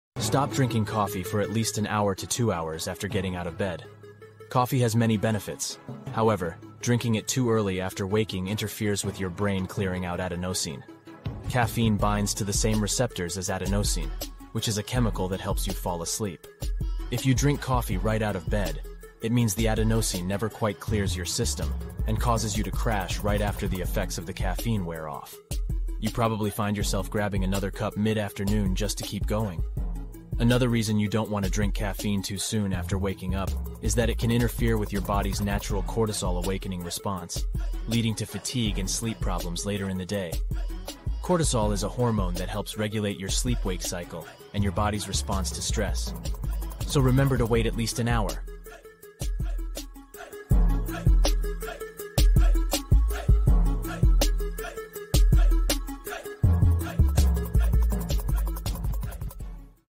Don't Drink Coffee, Check out why
Don't drink coffee for at least an hour to two hours after getting out of bed. Coffee has many benefits; however, drinking it too early after waking interferes with your brain clearing out adenosine. Caffeine binds to the same receptors as adenosine, which is a chemical that helps you fall asleep. If you drink coffee right out of bed, it means the adenosine never quite clears your system and causes you to crash right after the effects of the caffeine wear off. You probably find yourself grabbing another cup mid-afternoon just to keep going.